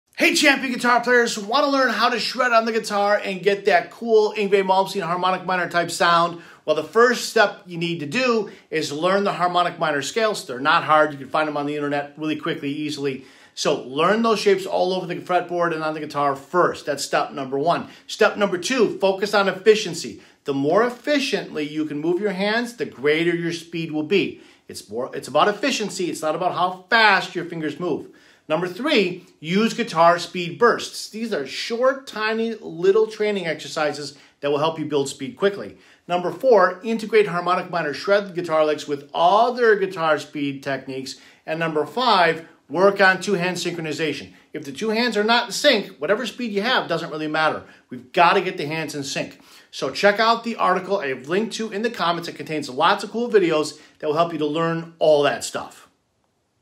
How To Shred On Guitar
Want to learn how to shred on guitar and get that cool Yngwie Malmsteen sound? Do this!